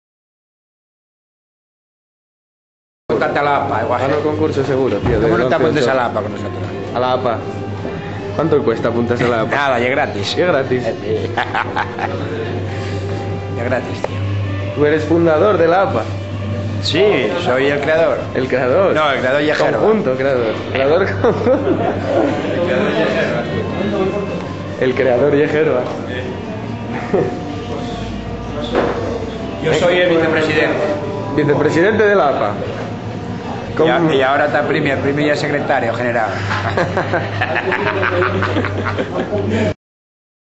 Apuntate a la APA
El Vicepresidente y cofundador de la APA nos invita a alistarnos a su asociacion.